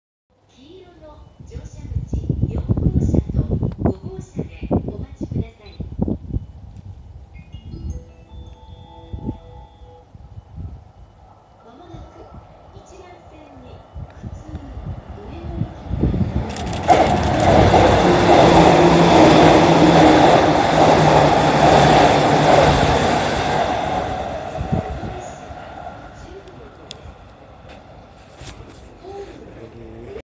651系 尾久駅通過
651系特急草津 尾久駅通過